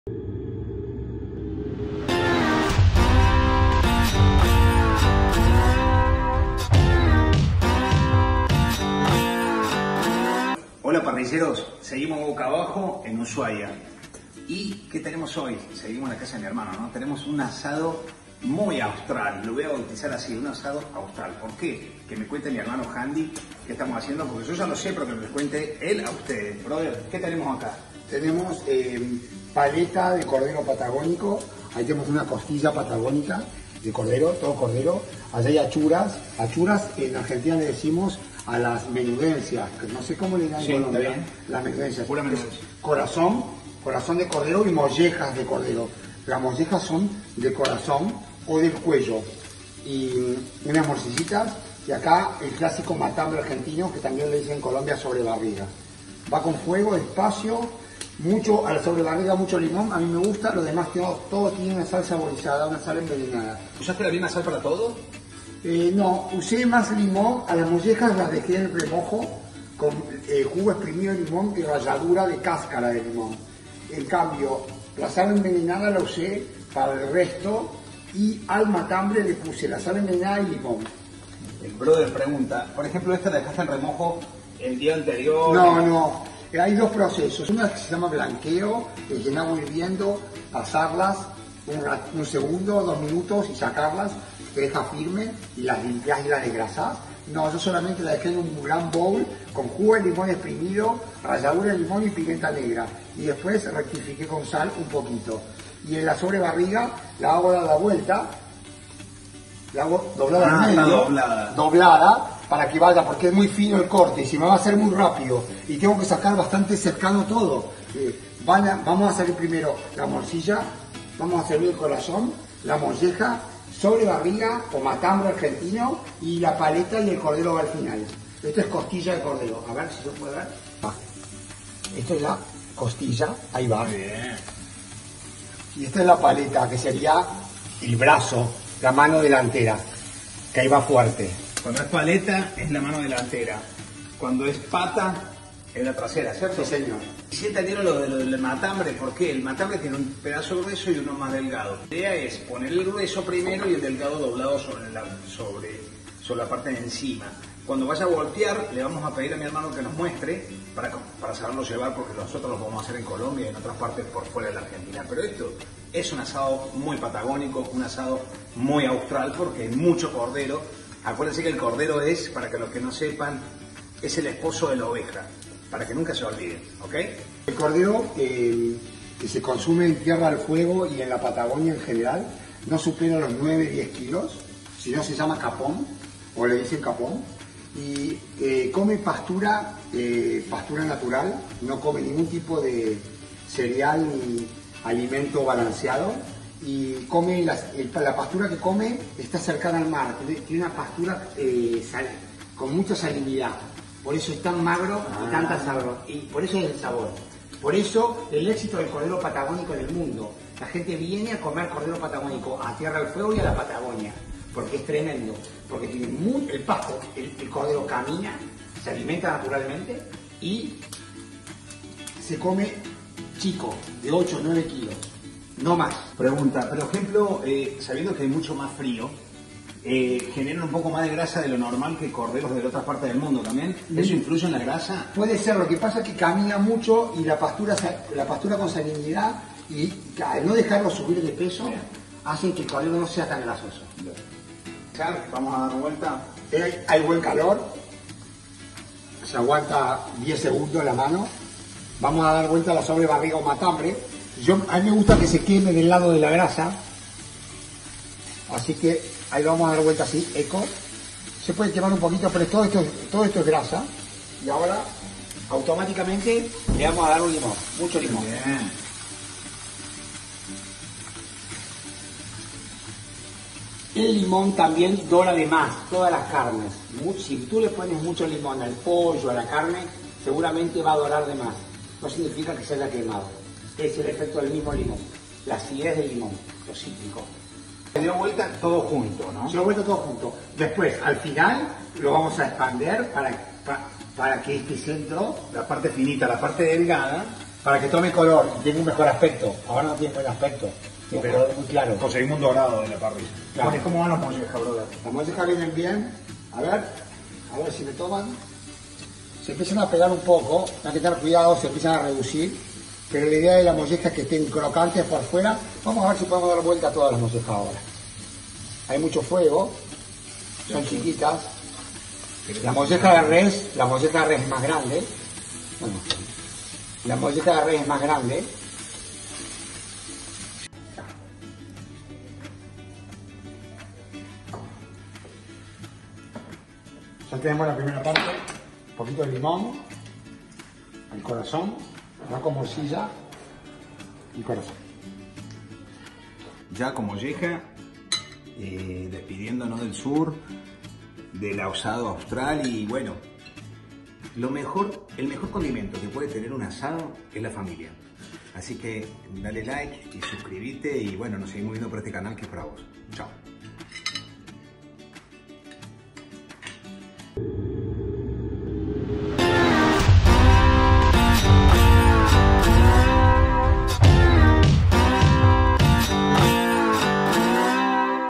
Cómo hacer un ASADO AUSTRAL | Comedero Kuipers | Estilo Kuipers Parrilla | Asados
Ya tenemos CURSO ONLINE a la parrilla.
¡COMPRA YA!

Hola parrilleros les cuento que estuve con mi hermano Jandy Kuipers, y nos preparó un asado austral en Ushuaia, y quiero que vean cómo vivimos esta experiencia.

Experiencia Kuipers Parrilla.